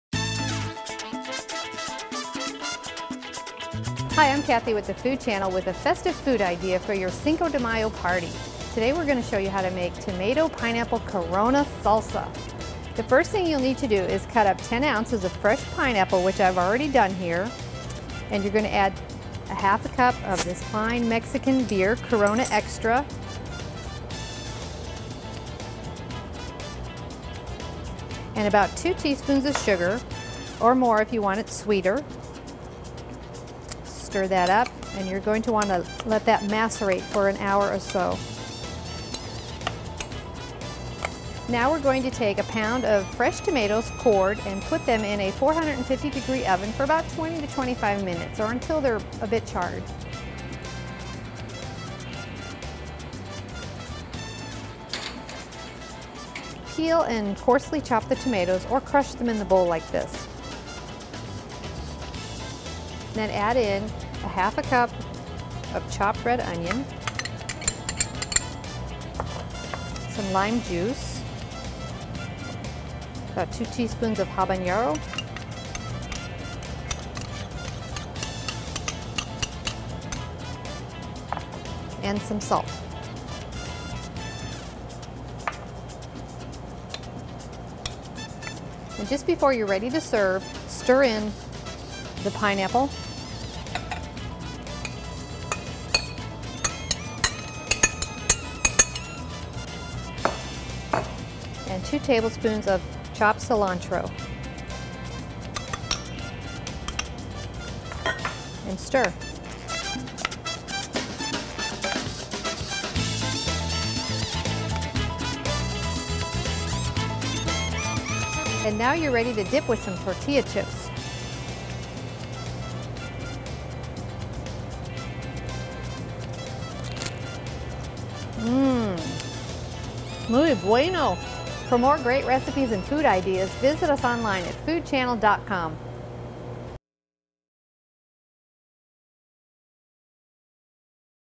How to Make Tomato Pineapple Corona Salsa
Here's a sweet and tangy salsa perfect for the warm weather or your Cinco de Mayo party! This salsa combines tomatoes, pineapple and the zing of Corona Extra. Try it! You'll love it! Kick up the heat if you dare by adding in some minced habañero chile!